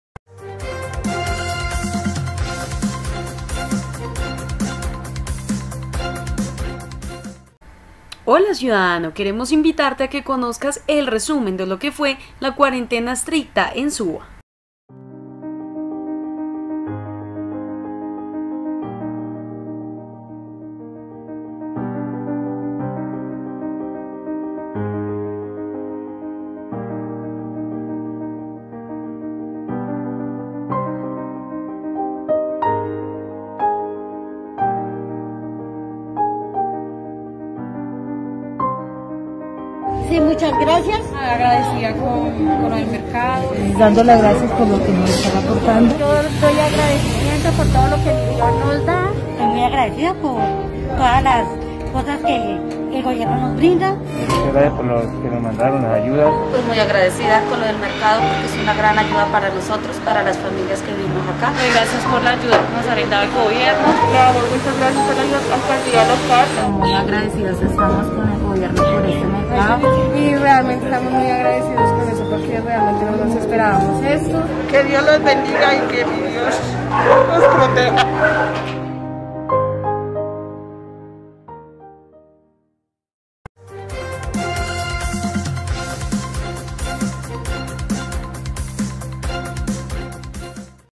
Conoce lo que fue la cuarentena estricta en Suba
Conoce as cifras que dejó la cuarentena estricta en la localidad de suba,La Alcaldía Local de Suba junto a Policía Nacional de los Colombianos y Secretaría Seguridad Bogotá realizaron un total de 3️⃣2️⃣8️⃣ operaciones de inspección, vigilancia y control a diferentes establecimientos de comercio y sectores de la localidad. ‬

Además los os gestores de seguridad de la Alcaldía Local de Suba llegaron a toda la localidad. Realizando pedagogía y sensibilización a más de 7.000 personas sobre los cuidados que debemos tener para prevenir la propagación del virus Covid-19. ‬